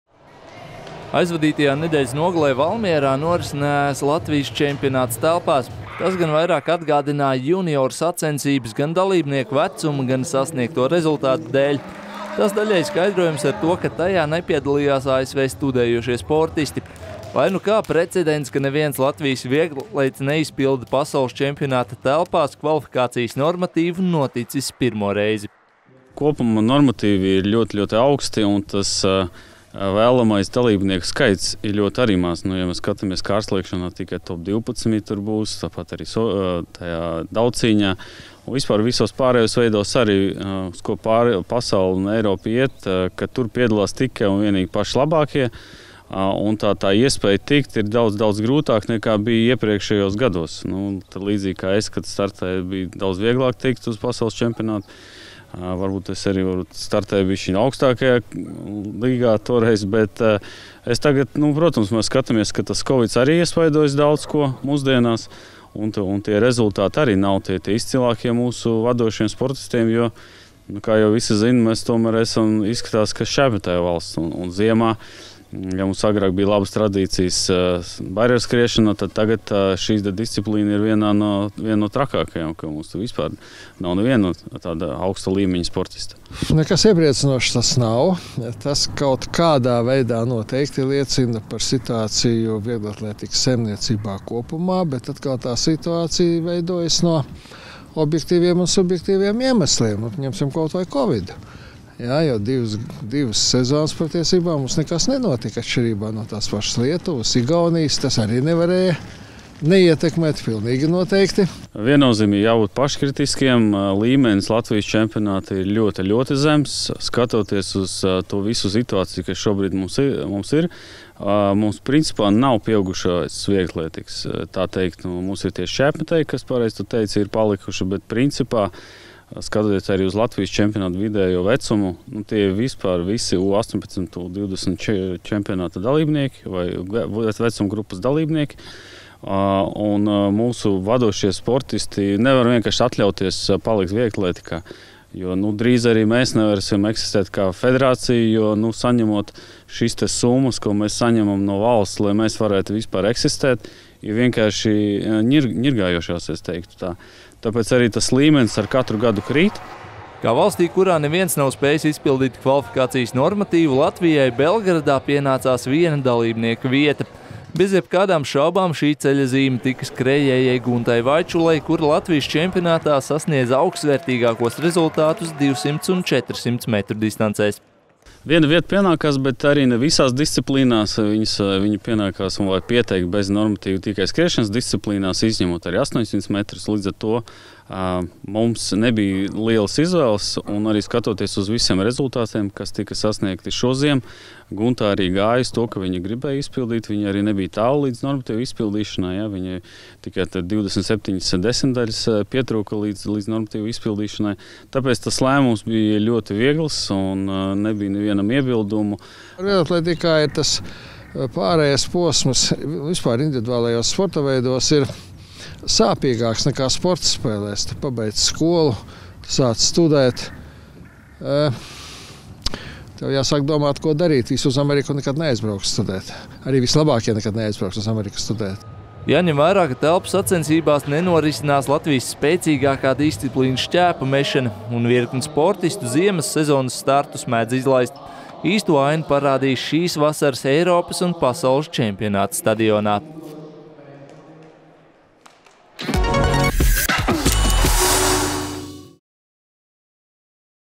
Latvijas vieglatlētu gatavība PČ vieglatlētikā telpās Serbijā
Pirmo reizi vēsturē neviens Latvijas sportists nav izpildījis normatīvu Pasaules čempionātam vieglatlētikā telpās. Latvijai piešķirto kvotu, kas pienākas valstīm, kurās neviens sportists nav spējis izpildīt normatīvu, izmantos 400m skrējēja Gunta Vaičule.

Pasaules čempionāts vieglatlētikā telpās Serbijā sāksies 18. martā.